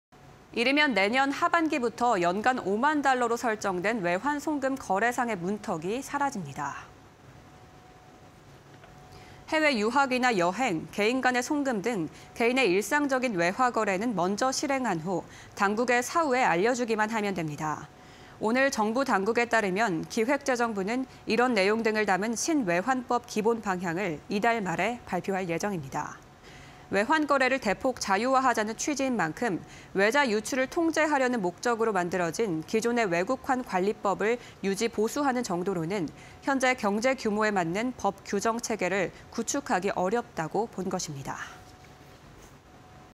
이르면 내년부터 연간 5만불 외화송금 문턱 규제 사라진다 (20230116)
이르면 내년 하반기부터 연간 5만 달러(미화)로 설정된 외환송금 거래상의 문턱이 사라집니다.

해외 유학이나 여행, 개인 간 송금 등 개인의 일상적인 외화 거래는 먼저 실행한 후 당국에 사후에 알려주기만 하면 됩니다.

오늘(16일) 정부 당국에 따르면 기획재정부는 이런 내용 등을 담은 신 외환법 기본방향을 이달 말 발표할 예정입니다.

외환 거래를 대폭 자유화하자는 취지인 만큼 외자 유출을 통제하려는 목적으로 만들어진 기존 외국환관리법을 유지·보수하는 정도로는 현재 경제 규모에 맞은 법·규정 체계를 구축하기 어렵다고 본 것입니다.